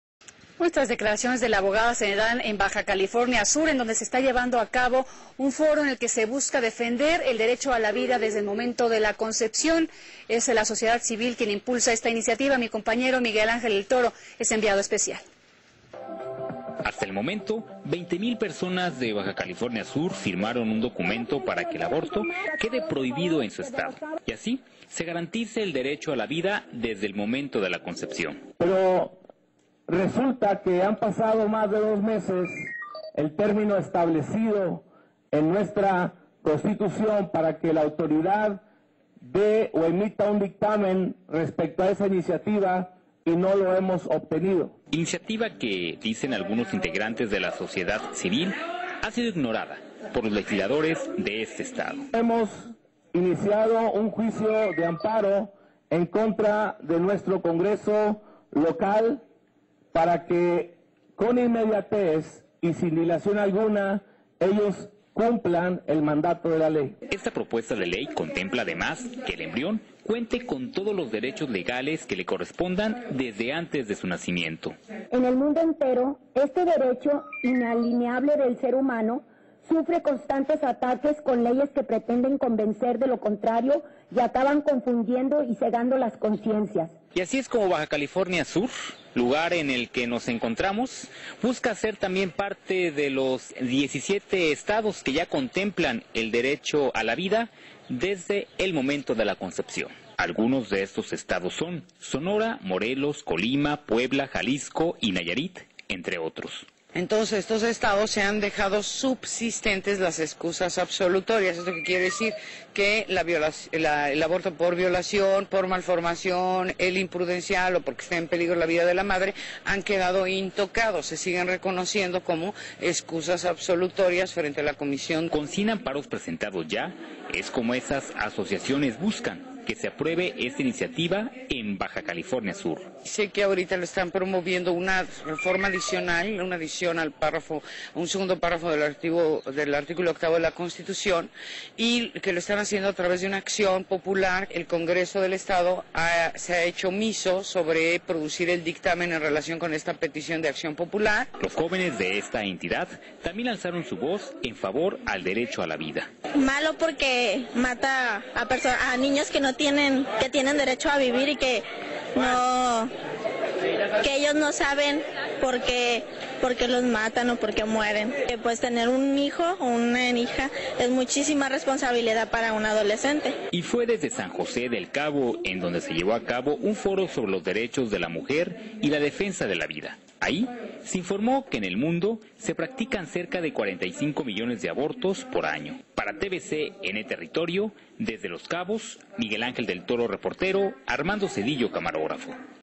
¡NO AL ABORTO EN BCS!
Nos oponemos terminantemente al aborto, tal y como lo proponen Narciso Agúndez Montaño, Luis Armando Díaz y la diputada abortista Graciela Treviño Garza, junto con el PRD de Baja California Sur. - Colectivo Pericú